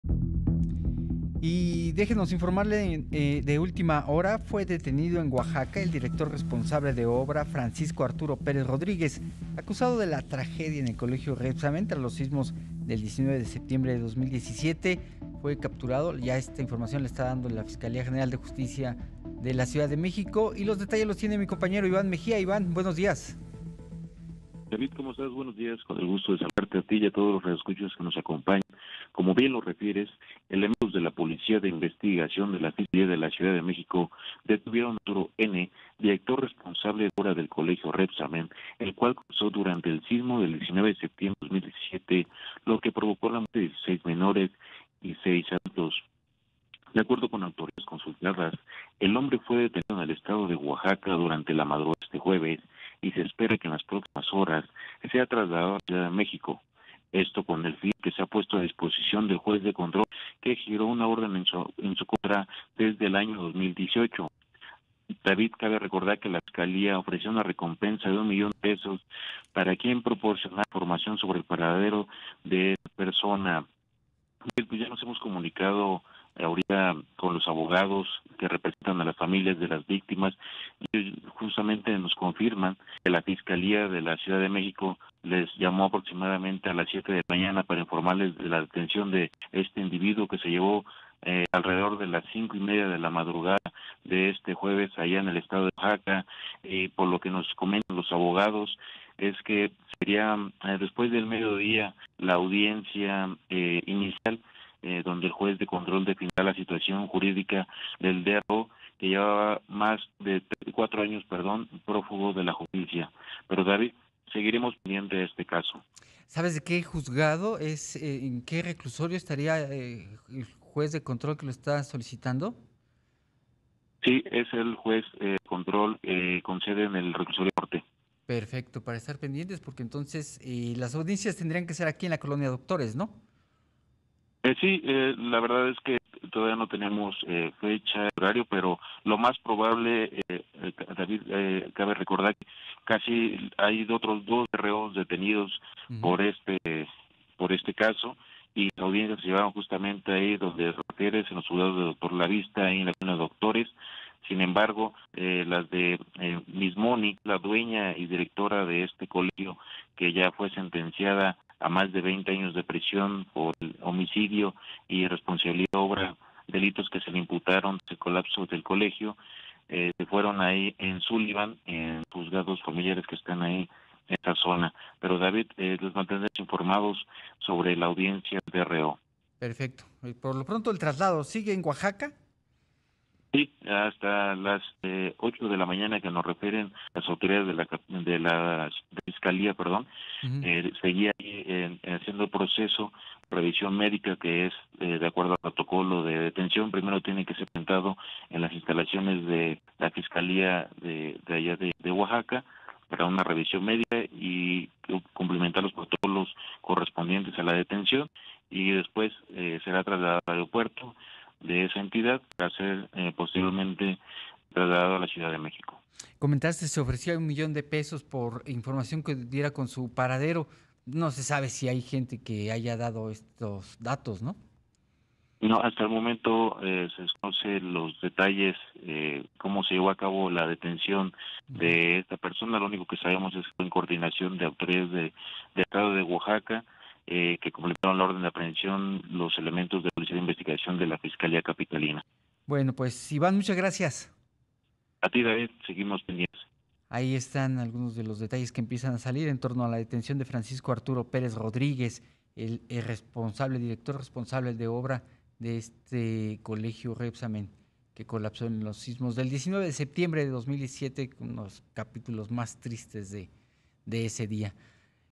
LO ÚLTIMO: Detienen en Oaxaca a director de obra del Colegio Rébsamen
El abogado Noé Franco, quien representa a un grupo de familiares de las víctimas del Colegio Rébsamen, confirmó la detención de Francisco Arturo Pérez, DRO que hacía falta por detener tras el colapso del inmueble que dejó 26 muertos.